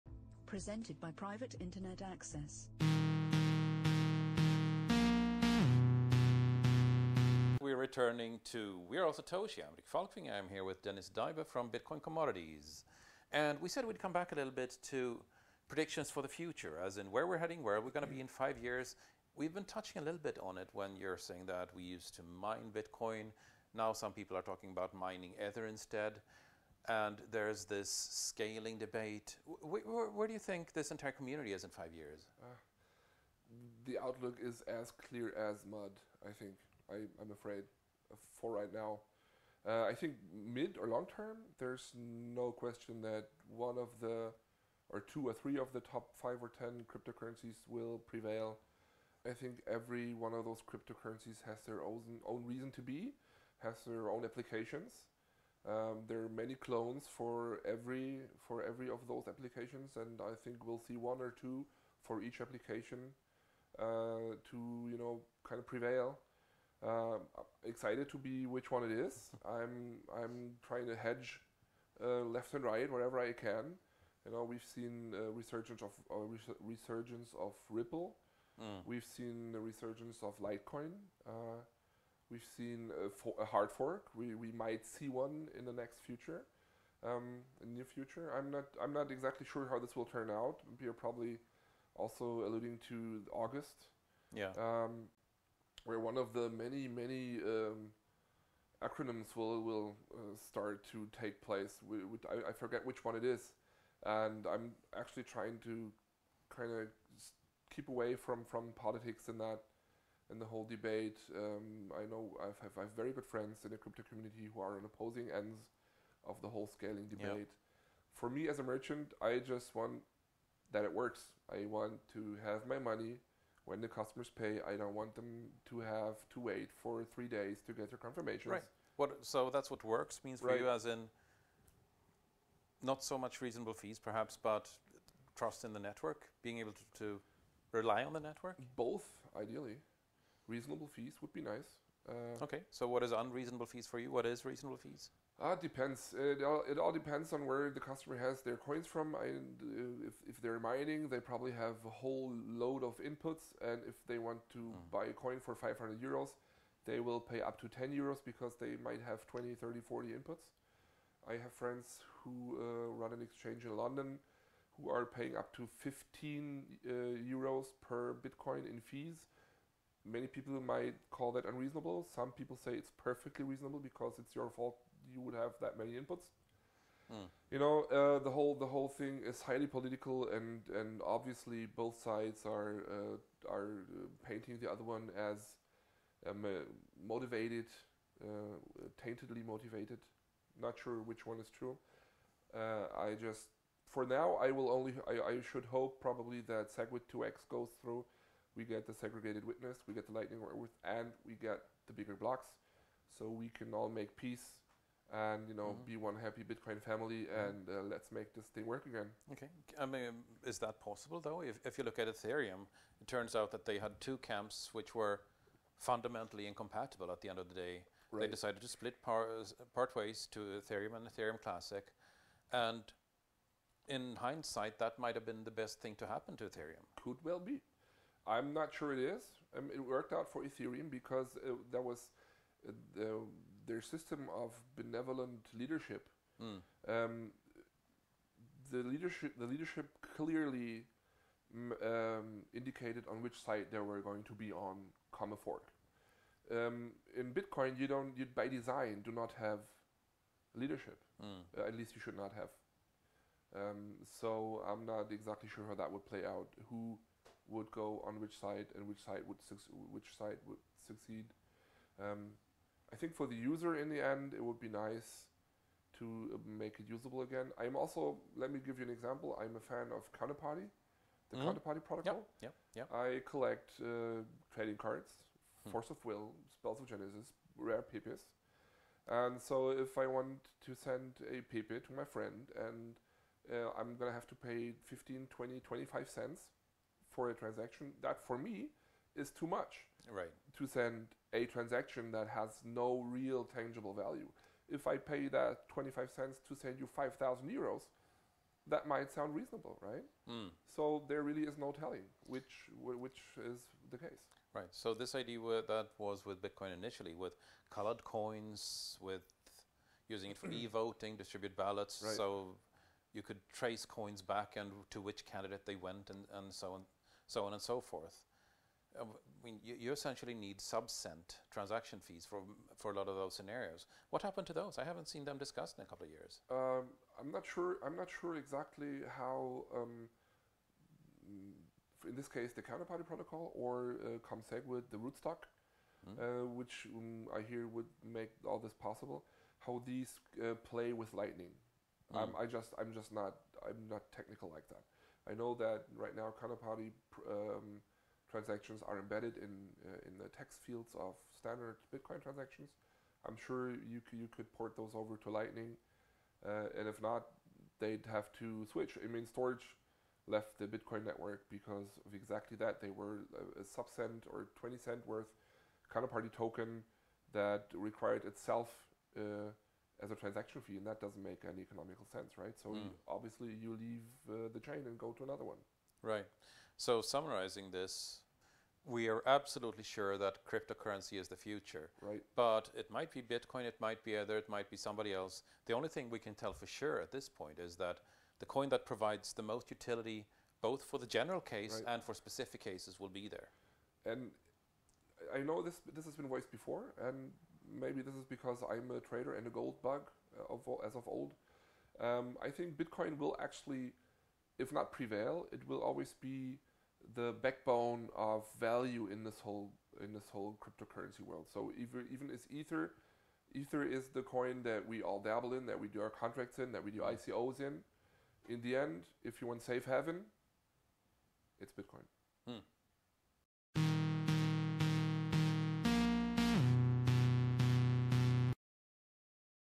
The future of Bitcoin with Dennis Daiber & Rick Falkvinge
We are all Satoshi. In our fourth episode, we meet Dennis Daiber from Bitcoin Commodities to talk honest money.

Dennis Daiber
Berlin, Germany

Equity Trader.

Owner of bitcoincommodities.com, delivering physical Gold and Silver for Crypto since 2011.

Consultant at Crypto Economic Consultant Group (cecg.biz). Advice and counsel on all things crypto, trading and cryptotrading.

Other interests include poker, whisky, BBQ and M:tG.